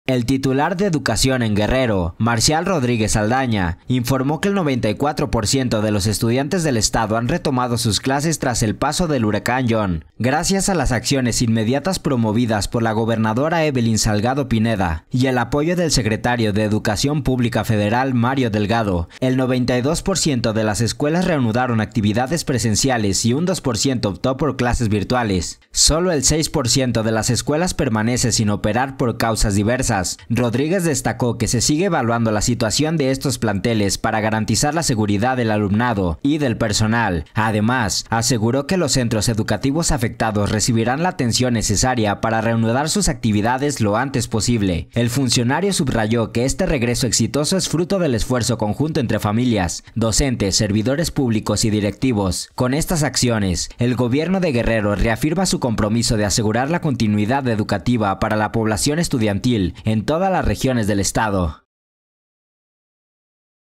Vuelven a clases 94% de los estudiantes de Guerrero tras paso del huracán ‘John’
El titular de educación en Guerrero, Marcial Rodríguez Saldaña, informó que el 94% de los estudiantes del estado han retomado sus clases tras el paso del huracán John. Gracias a las acciones inmediatas promovidas por la gobernadora Evelyn Salgado Pineda y el apoyo del secretario de Educación Pública federal, Mario Delgado, el 92% de las escuelas reanudaron actividades presenciales y un 2% optó por clases virtuales.